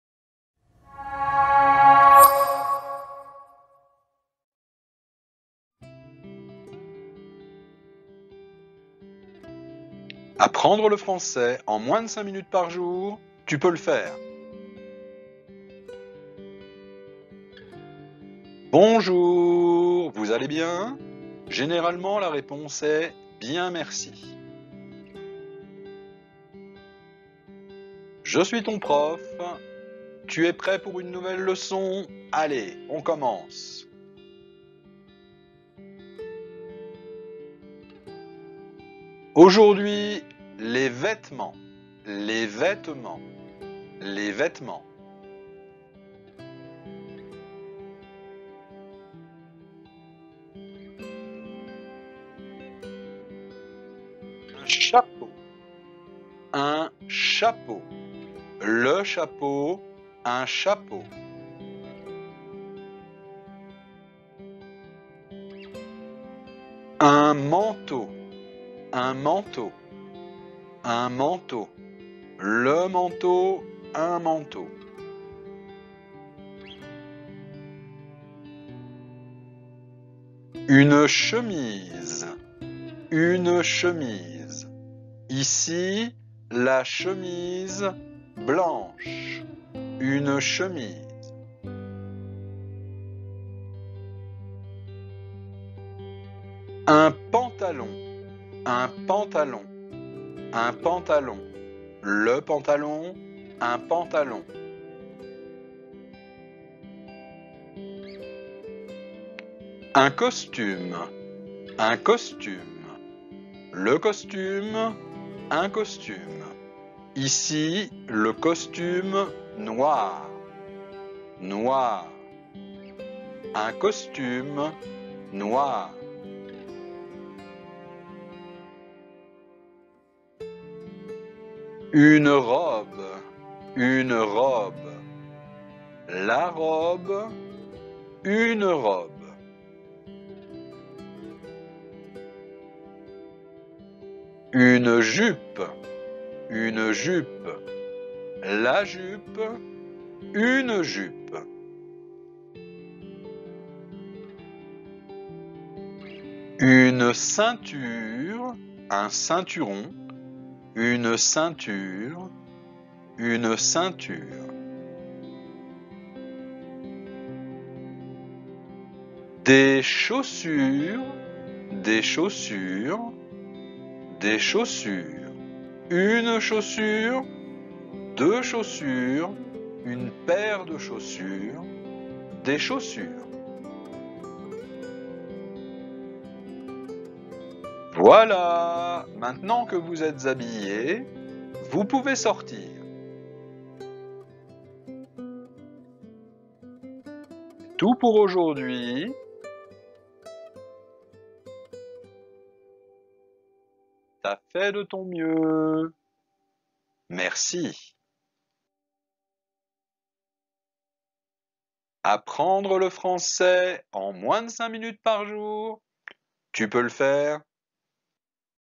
Aprende frances en menos de 5 minutos N#8
Esta serie de videos esta dedicada a todas las personas que quieran aprender a hablar francés